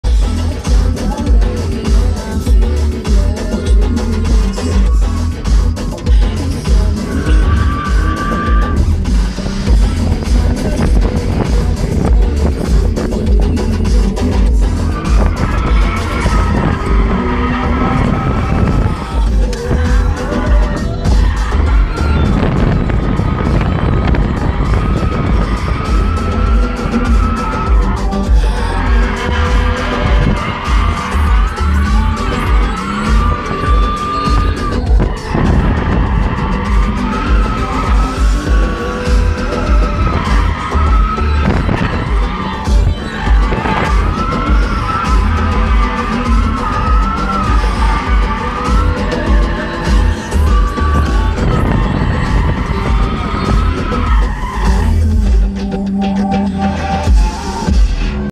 2023 BMW M240i XDrive ... Drifting? Yep!
NOT XDELETE!
XDelete required pre-june-21-ish DSC module which was upgraded in the middle of iDrive7. Some 2022s can work. NONE of the iDrive8 ('panoramic screen') models will support it... Possibly ever... I'll go into detail soon.